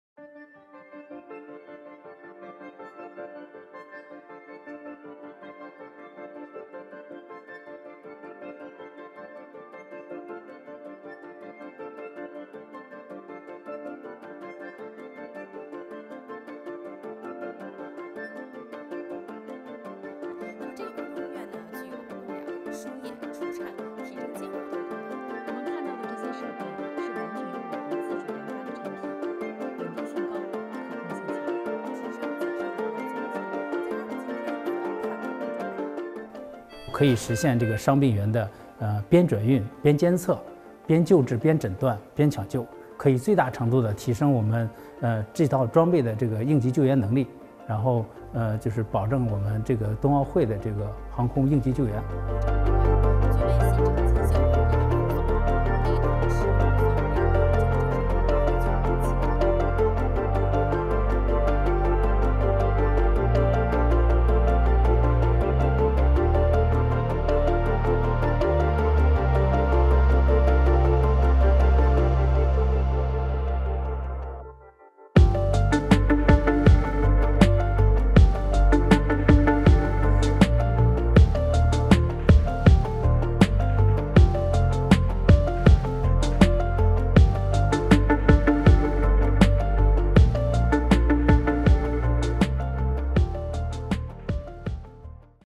China’s air ambulance system handles Winter Olympics emergency transport of injured athlete
Described as airborne hospitals, China’s helicopter ambulance service has been put into action to handle an emergency rescue during the Beijing 2022 Winter Olympic Games.  China set up the emergency medical transport system to quickly get help to injured or sick competitors during the Games. On February 7, 2022, the air ambulance and crew cared for an athlete injured at the Olympic venue in Zhangjiakou during a 20-minute flight to a dedicated hospital in Beijing.